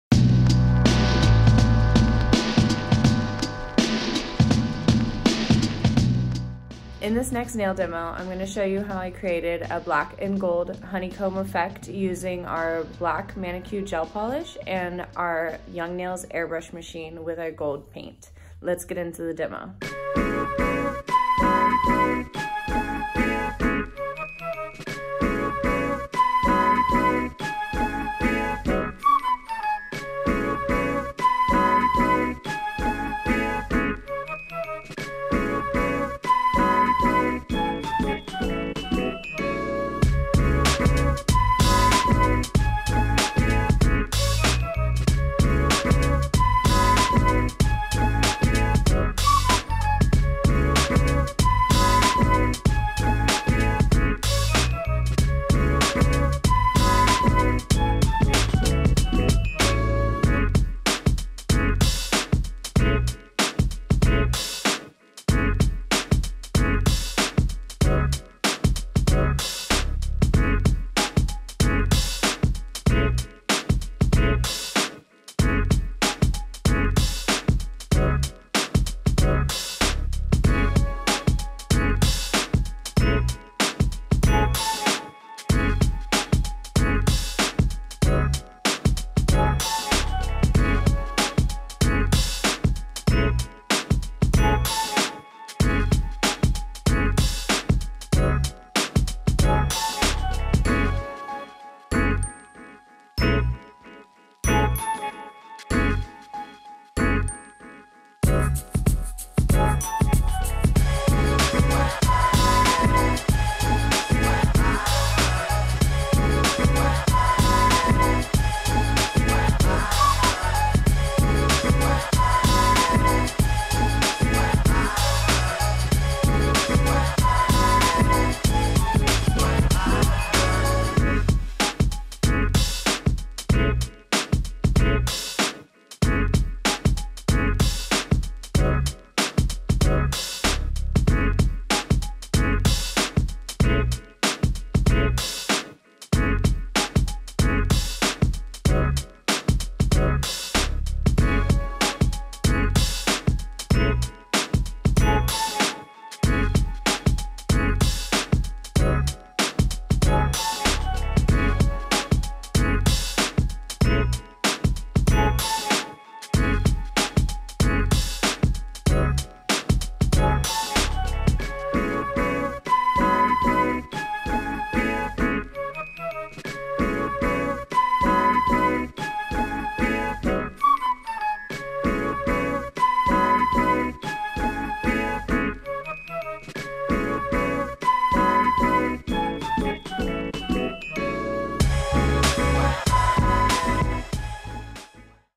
Young Nails Nail Demo - Honeycomb Nail Design - Gel Polish/Airbrush Nails
Watch as Stephanie Fountain (@glossboss23) creates this awesome Honeycomb design using Mani Q Black 101 and airbrushing gold.